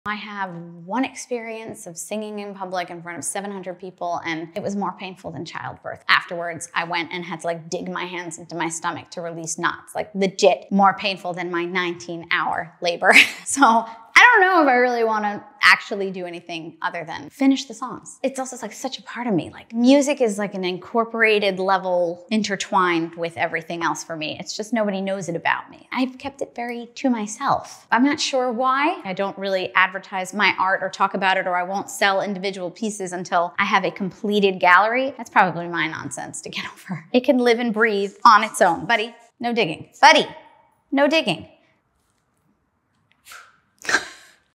The Entrepreneur Mindset
Have you ever had an idea that you just can't get out of your head?

Something that you know needs to be brought to life?

That's how I feel about making music. (And art…focus Shira.)

I don’t want to be a singer.

I'm just consumed with the innate need to make it a reality.

It's frustrating to have a song in my head that needs to come out 😩 I don’t play any instruments well, which makes it even more challenging to convey properly and get help completing them.

Either way, until I actually push myself to finish & record my songs…I’ve shared the music that totally feeds my soul, mood & motivation.

I call it the ”Bad B Playlist”. Linked at the top of my profile!